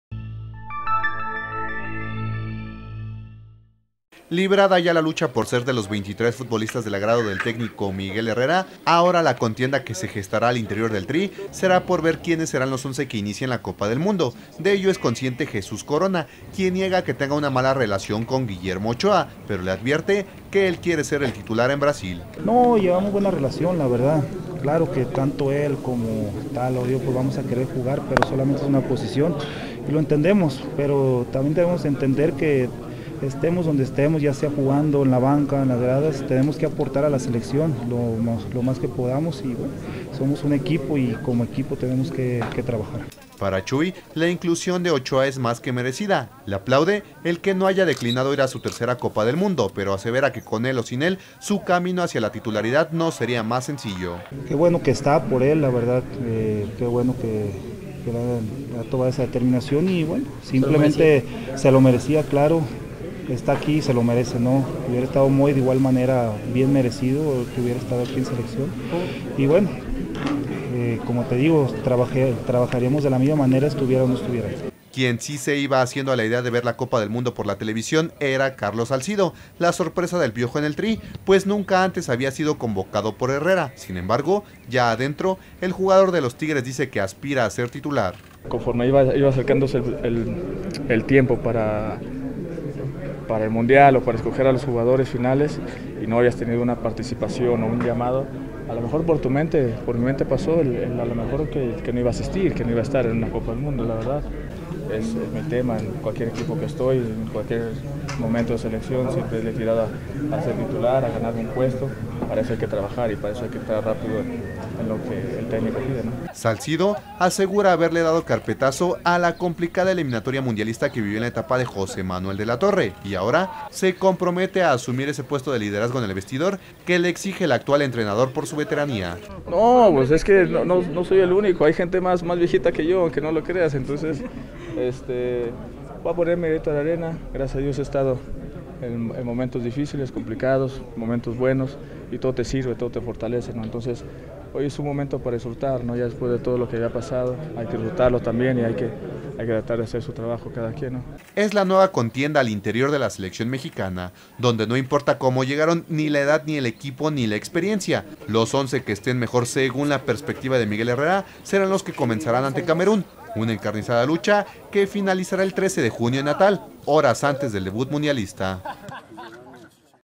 Corona, listo para pelear la portería con Ochoa
BRASIL 2014. México, DF. 15/05/2014. Librada ya la lucha por ser de los 23 futbolistas del agrado del técnico Miguel Herrera, ahora la contienda que se librará al interior del Tri será por ver quienes serán los once que inicien la copa del mundo.